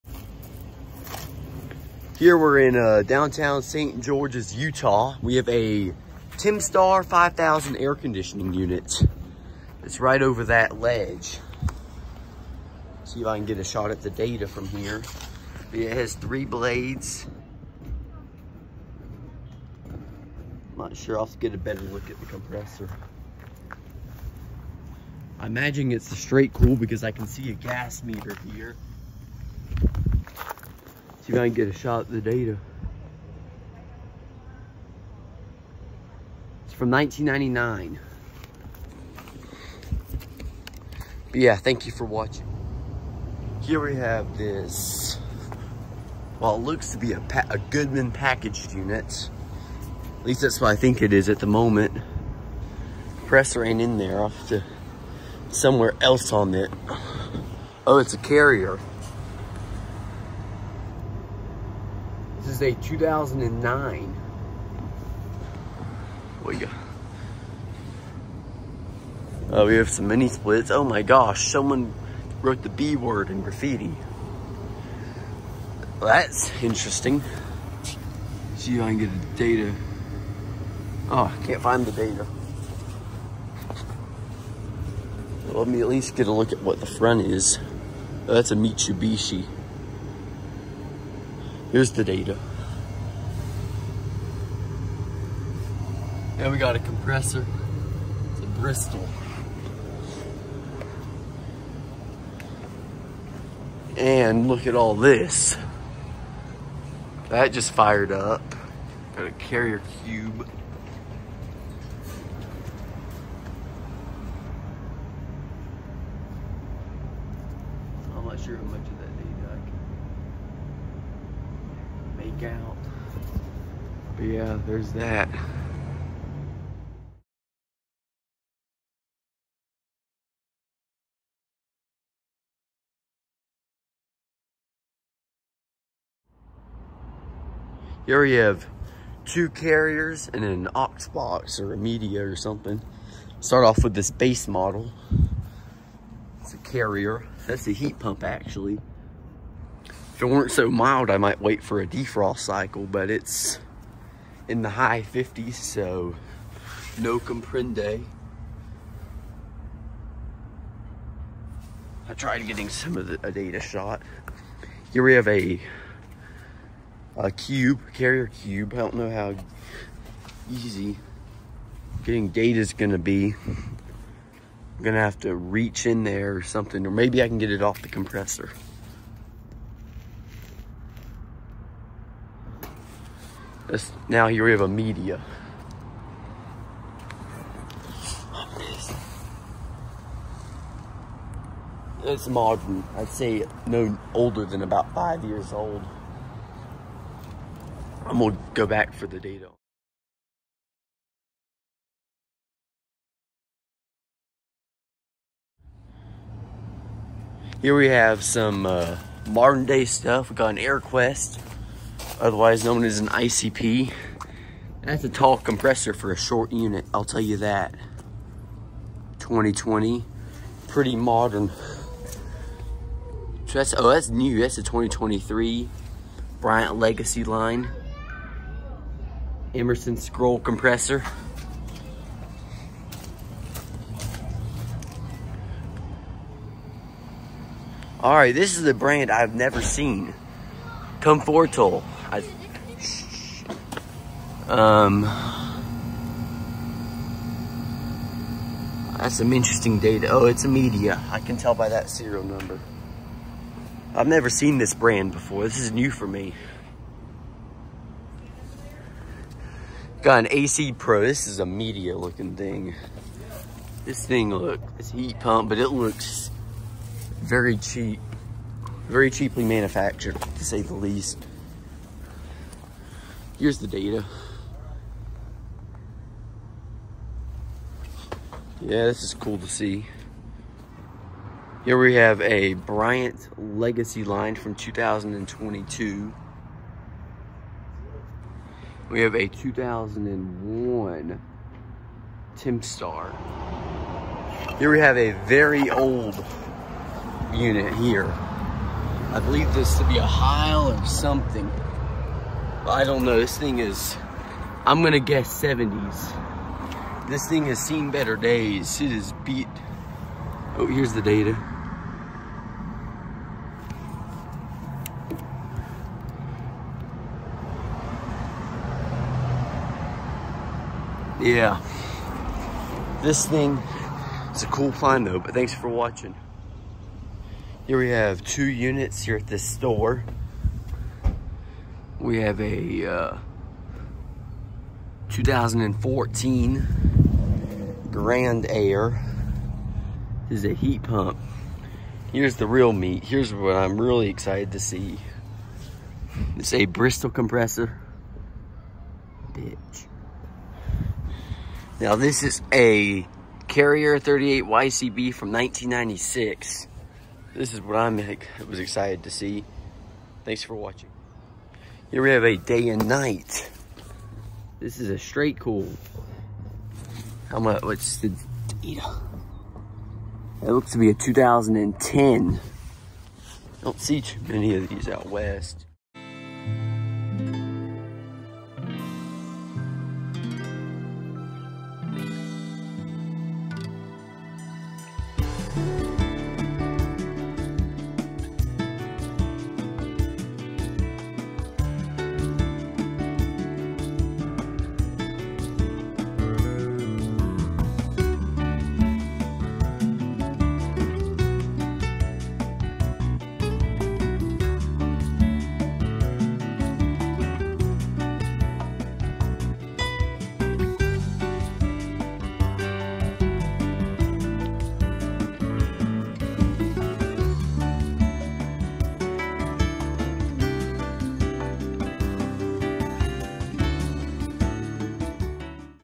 Vintage 1975 DNP Cool Cube Air Conditioner + HVAC Heaven in St. George, Utah
Enjoy! Located in St. George, Utah. This 1975 DNP Cool Cube is now my oldest unit up to date

Time Stamps:
0:00 1999 Heil 5000 Air Conditioner